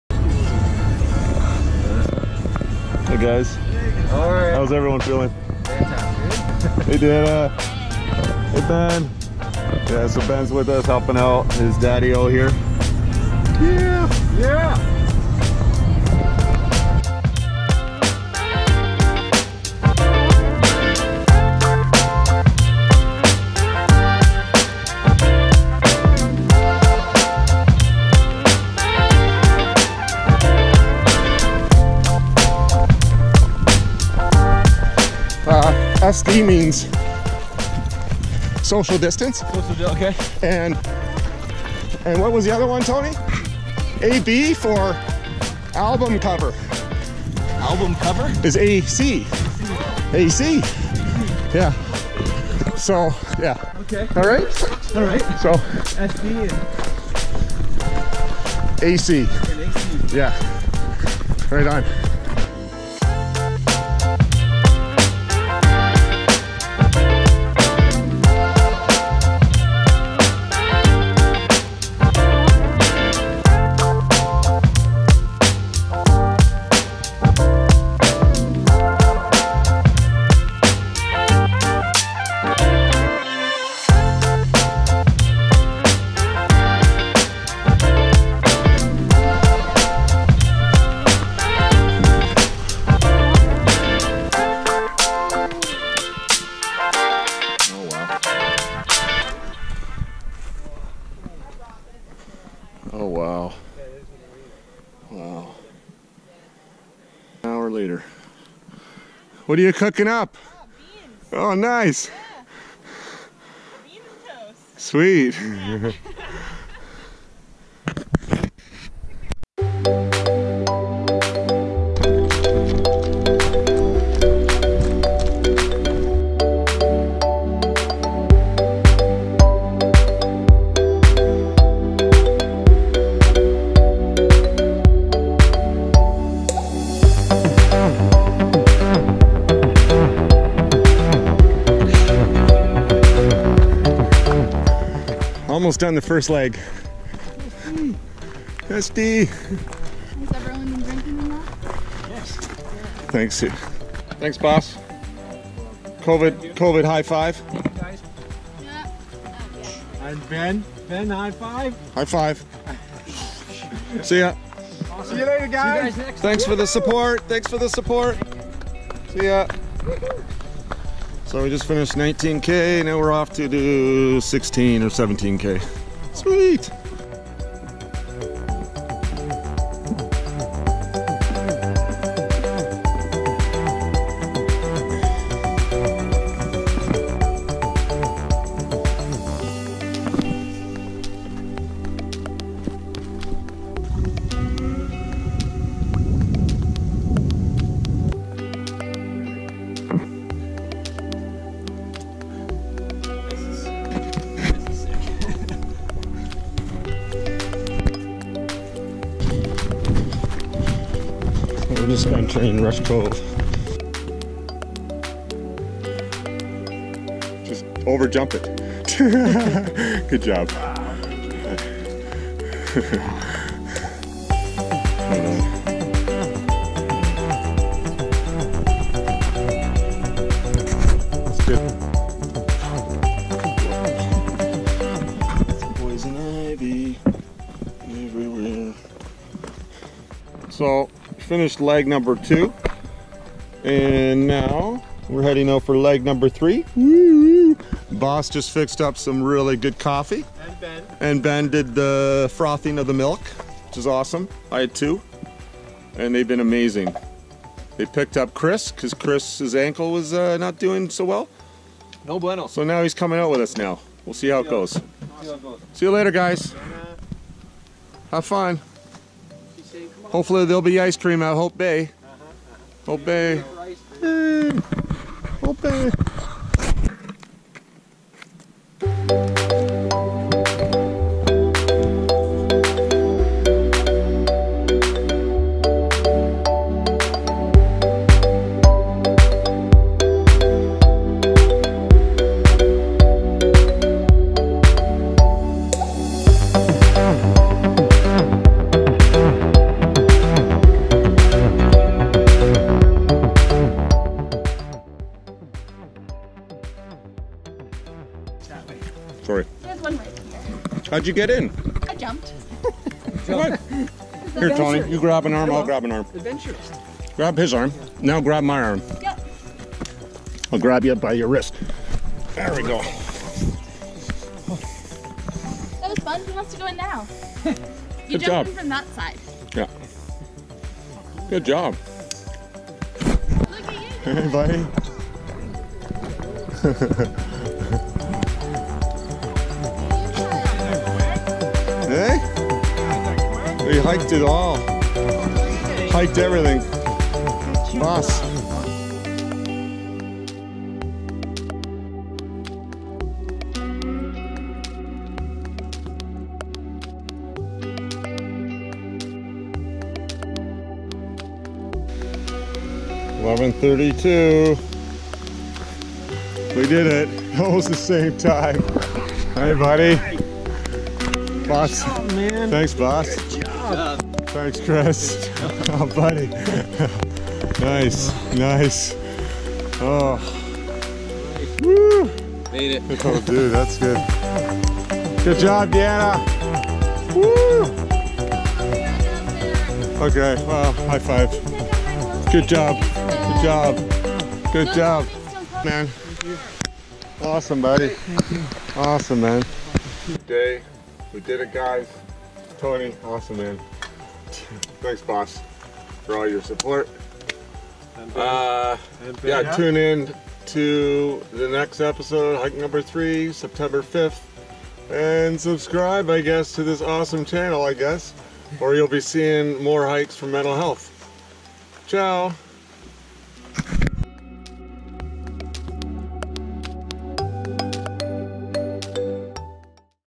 2nd Hike for Mental Health - Hike, Bike, Run Episode 4
August 8th 2020, Our second hike for Mental Health had us going another 75km. we started North of Lions Head around 7:30am and went down to Wiarton finishing around 11:30pm.

I had Tony Hrycyna, Deanna Brasz and Chris Oates hiking along side me. We also had Bastiaan Goedhart and his son Ben support us along the way.